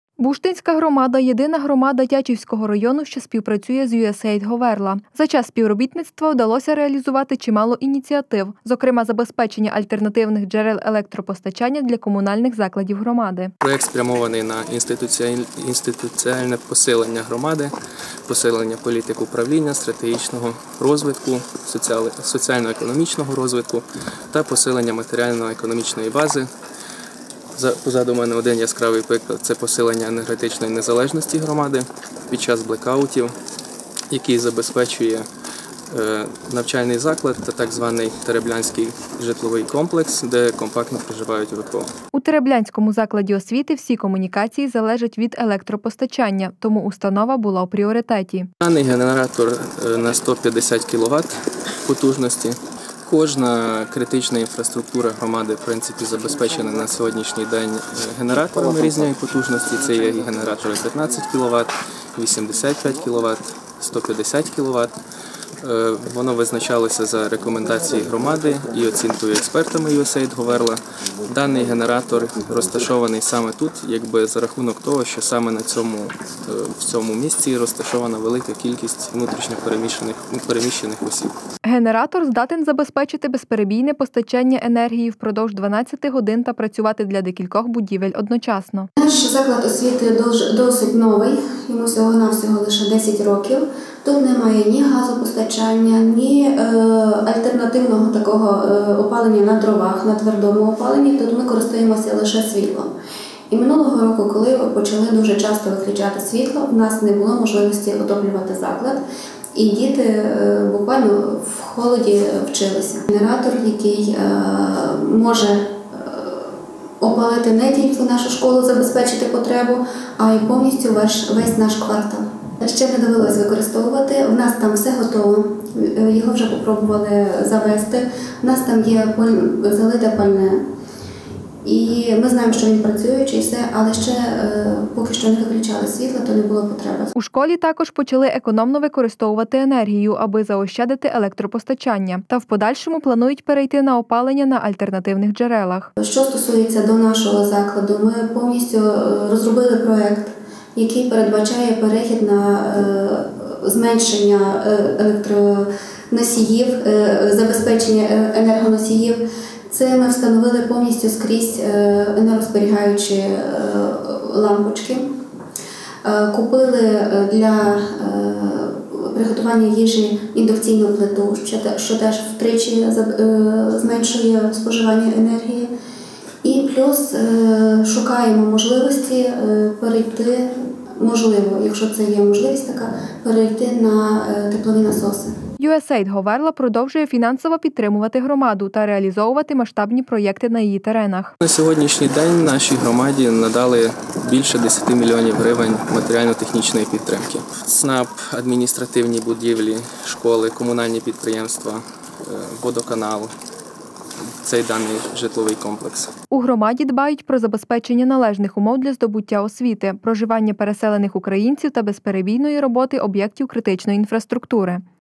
Альтернативні джерела електропостачання
Буштинська громада  - єдина громада Тячівського району, що співпрацює з USAID «ГОВЕРЛА».  За час співробітництва вдалося реалізувати чимало ініціатив, зокрема забезпечення альтернативних джерел електропостачання для комунальних закладів громади.  У Тереблянському закладі освіти – всі комунікації залежать від електропостачання – тому установа була у пріоритеті.  Генератор здатен забезпечити безперебійне постачання енергії впродовж 12-ти годин та працювати для декількох  будівель одночасно.  У школі також почали економно використовувати енергію, аби заощадити електропостачання. Та в подальшому планують перейти на опалення на альтернативних джерелах.  USAID «ГОВЕРЛА» продовжує фінансово підтримувати громаду, та реалізовувати масштабні проєкти на її теренах.  У громаді дбають про забезпечення належних умов для здобуття освіти, проживання переселених українців та  безперебійної роботи об’єктів критичної інфраструктури.